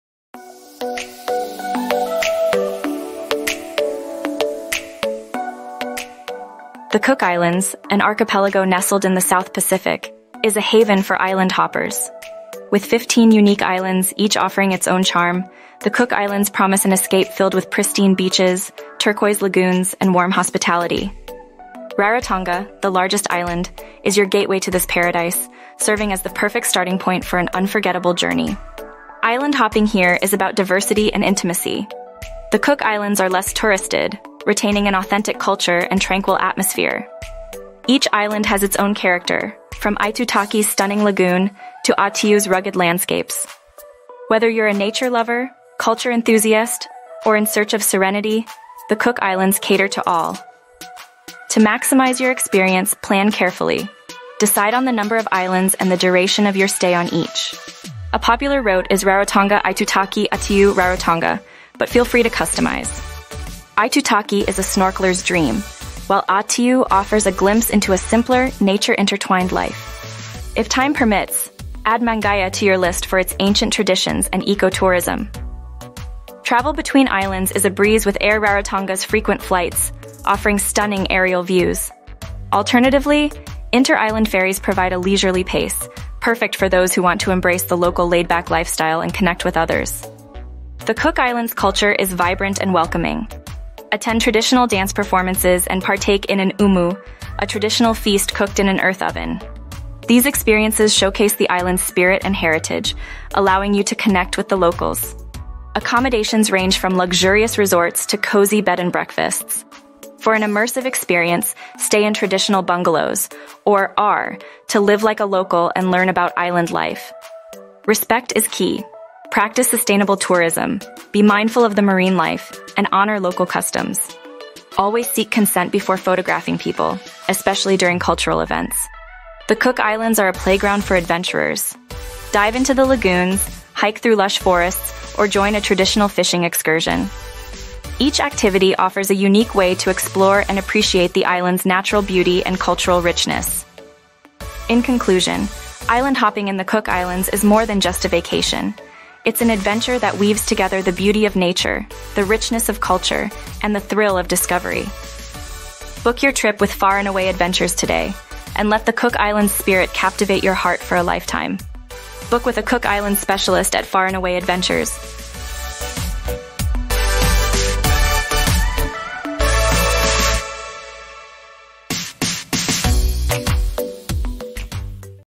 The Ultimate Guide to Island-Hopping in the Cook Islands
Discover the magic of island-hopping in the Cook Islands, a picturesque archipelago in the heart of the South Pacific. Perfect for a tropical escape, the Cook Islands offer pristine beaches, turquoise lagoons, and warm hospitality. Join us as we delve into the wonders of island-hopping in this paradise, providing you with all the essential information to make your journey unforgettable. Explore everything you need to know to experience the unique and unparalleled beauty of the Cook Islands.